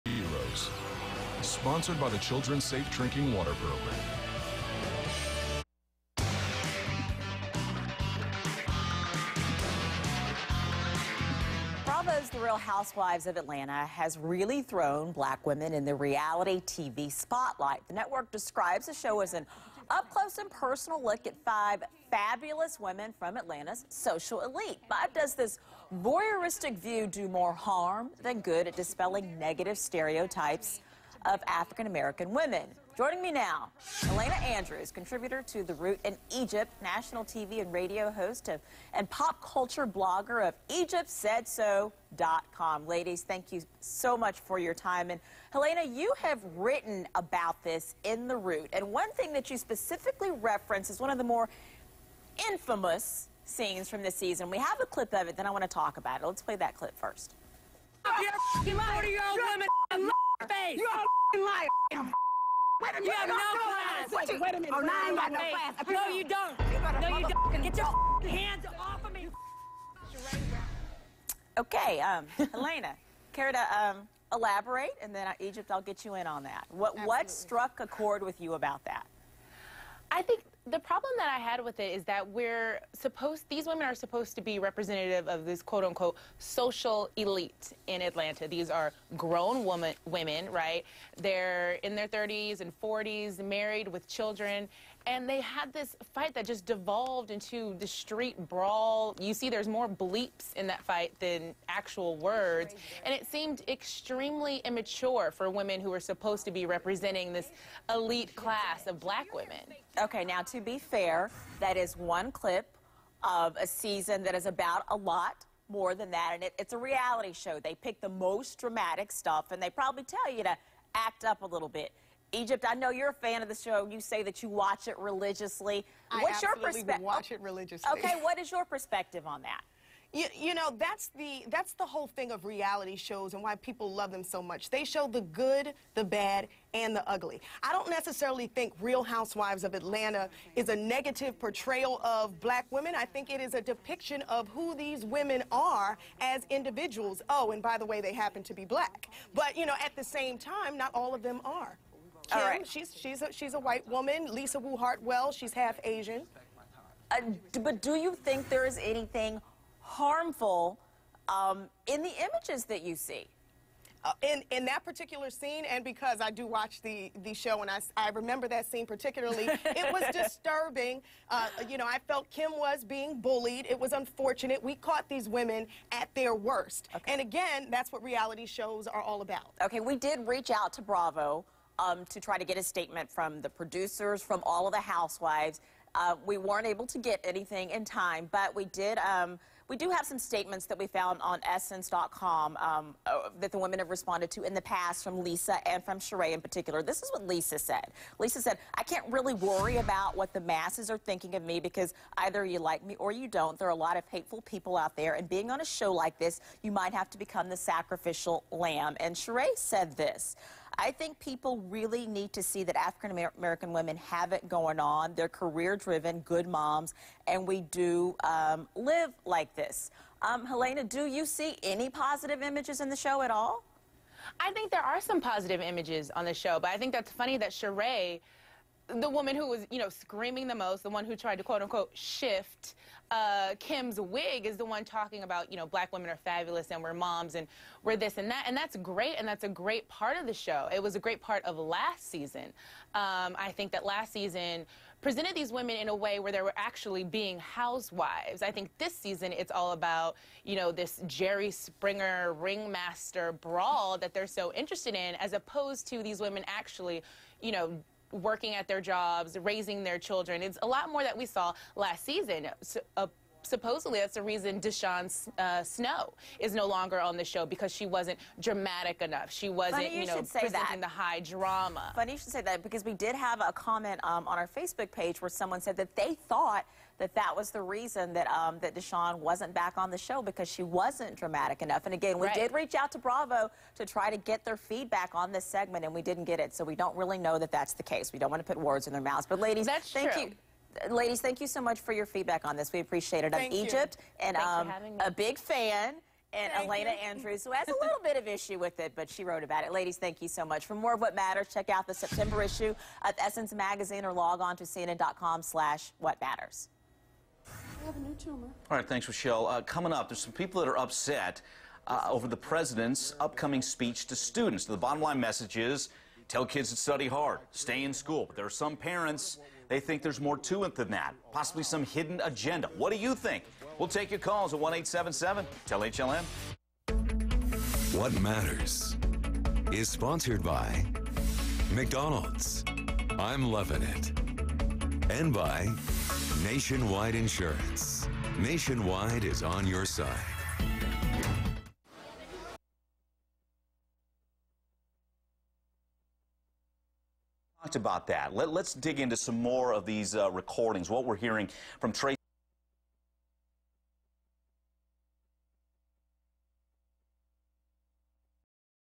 EGYPT ON CNN
TV AND RADIO PERSONALITY EGYPT SPEAKS ON CNN ABOUT REAL HOUSEWIVES OF ATLANTA